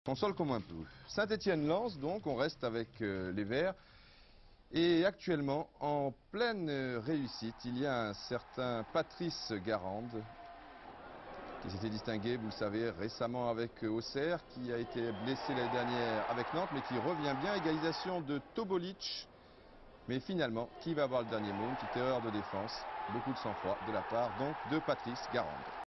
ASSE 2-1 Lens - 9e journée de D1 1987-1988
Mercredi 2 septembre 1987

Voici une belle victoire de ces pétillants Verts de 87-88 face au RC Lens qui vit une saison difficile (il sera relégué en fin de saison suivante).

L'attaquant Patrick Garande offre la 3e victoire à domicile de la saison, sur pénalty puis sur une erreur du futur capitaine vert Jean-Guy Wallemme. Il revit sous le maillot du club qui l'a formé, 10 ans avant. Il finira meilleur buteur du club avec 17 réalisations avant d'être progressivement écarté par Herbin la saison suivante, malgré un dernier doublé... à Lens Il jouera ensuite une saison chez ses victimes Sang et Or...

Les Stéphanois sont 7es.

Ils avaient vaincu un peu plus tôt le FC Metz d'un autre de leur futur capitaine, Sylvain Kastendeuch, auteur d'un contre son camp :

Images TF1